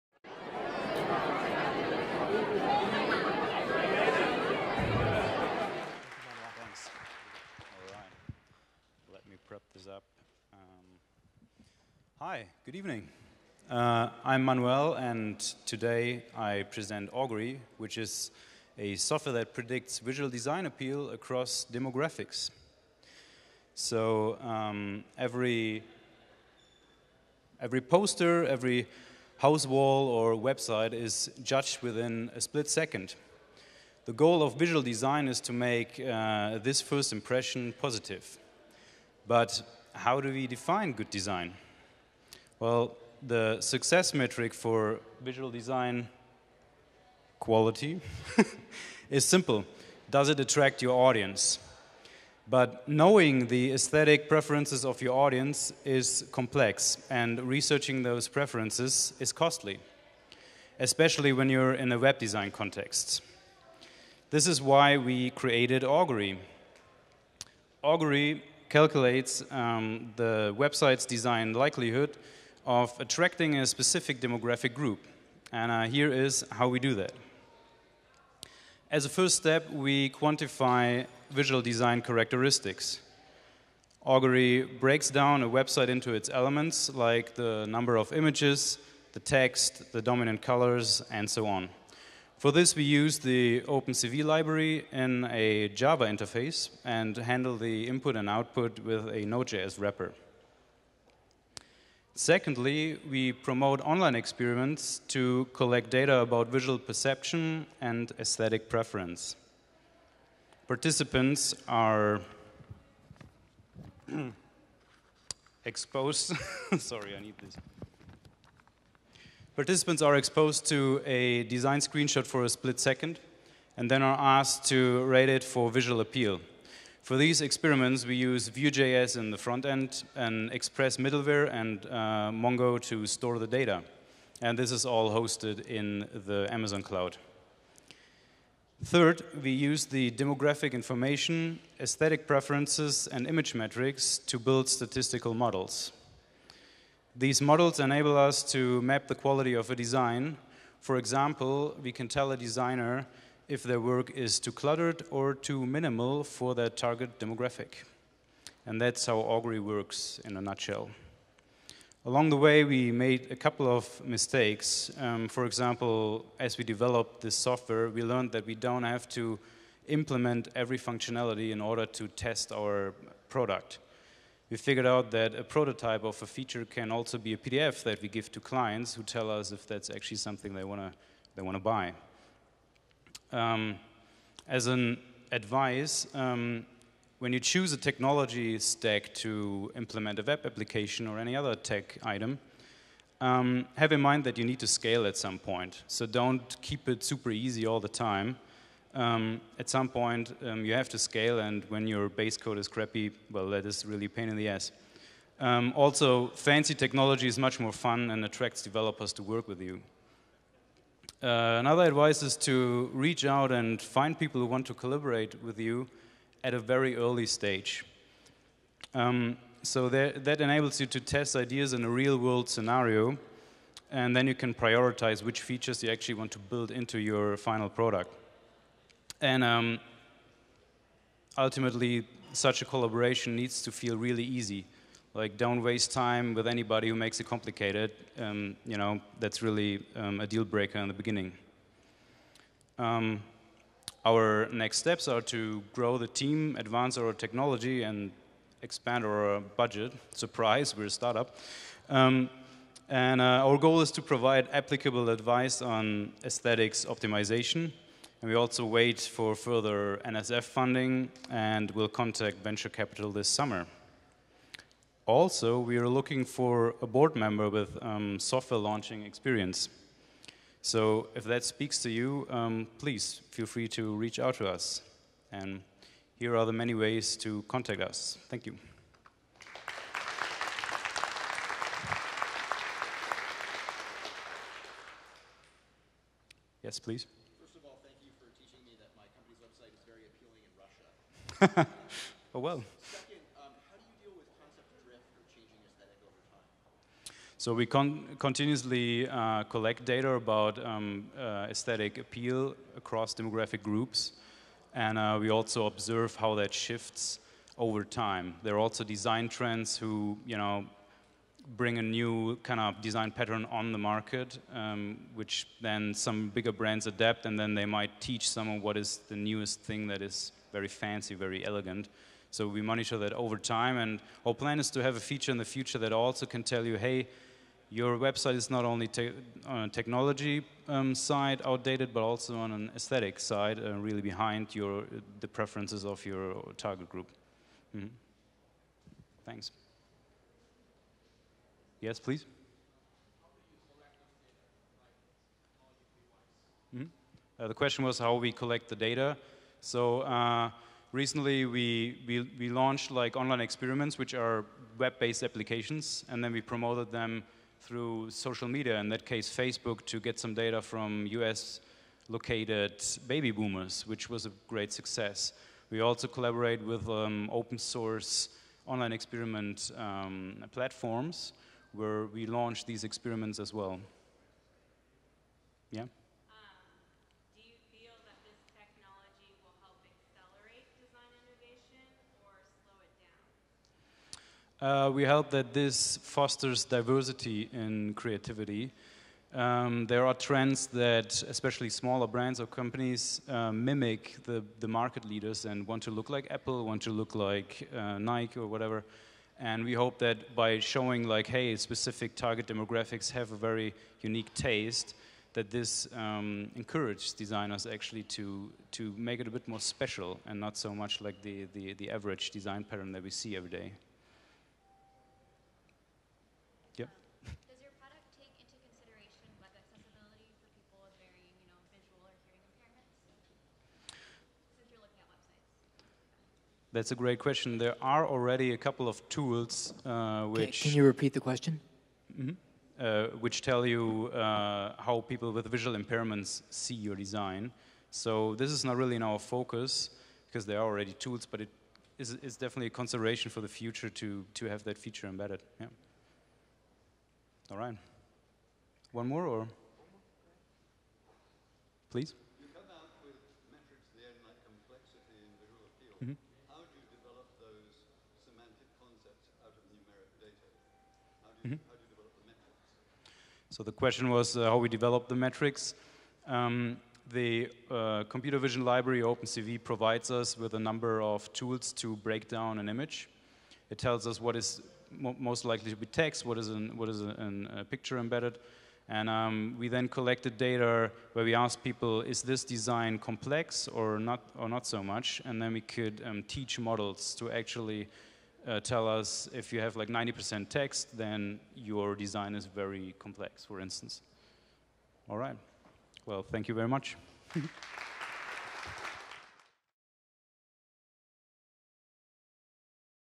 Augury
Augury presented by Katharina Reinecke and Manuel Nordhoff  on May 14, 2019 at New Tech Seattle at our 7th annual UW event.